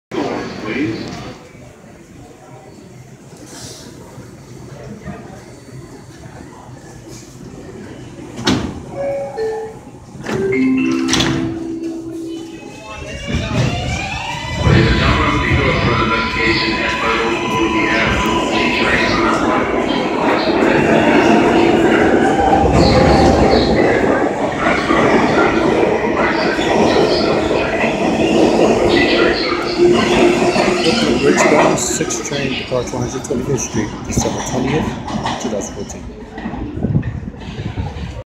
Brooklyn Bridge Bound R142A 6 train Departs 125th Street
Title says it all. December 20th 2014